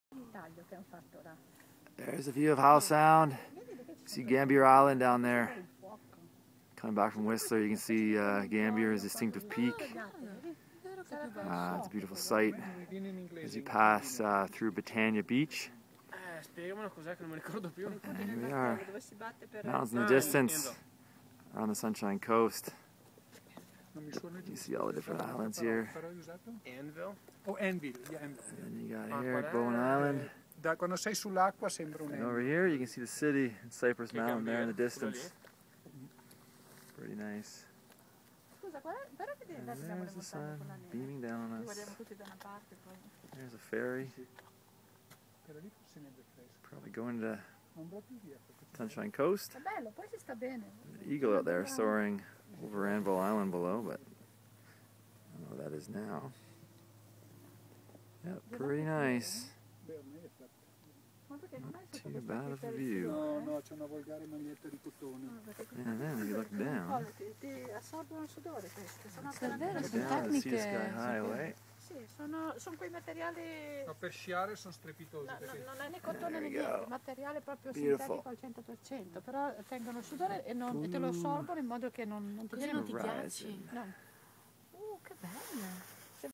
St Marks Summit: Sparkling Waters of Howe Sound
The view from the second peak of St Mark's Summit, high above Howe Sound and the Sea To Sky Highway.